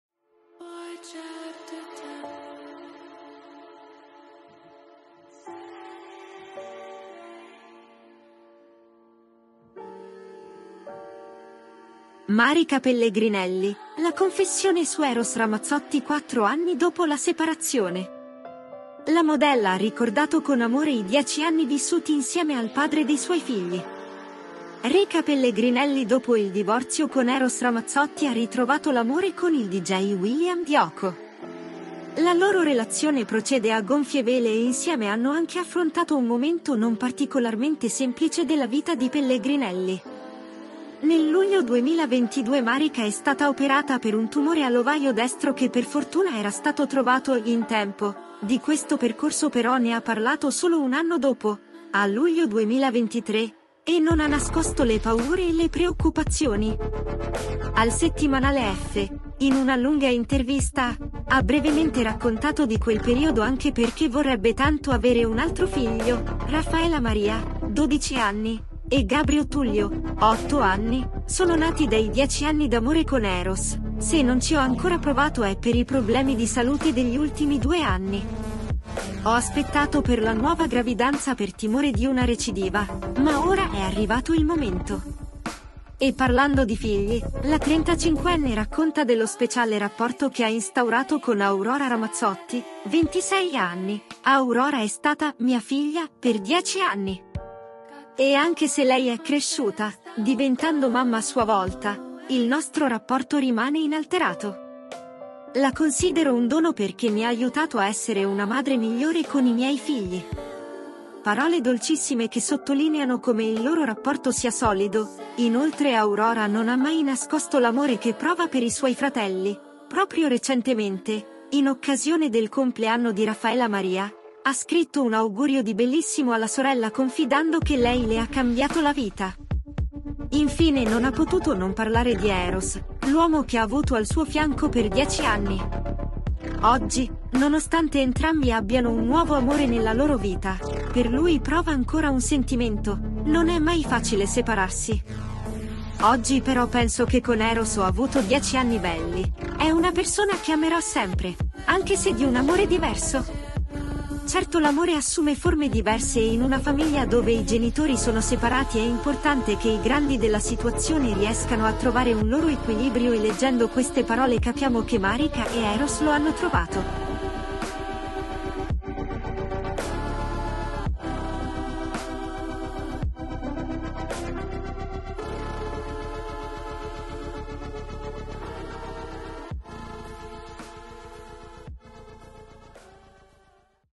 Marica Pellegrinelli, la confessione su Eros Ramazzotti quattro anni dopo la separazione
La modella ha ricordato con amore i 10 anni vissuti insieme al padre dei suoi figli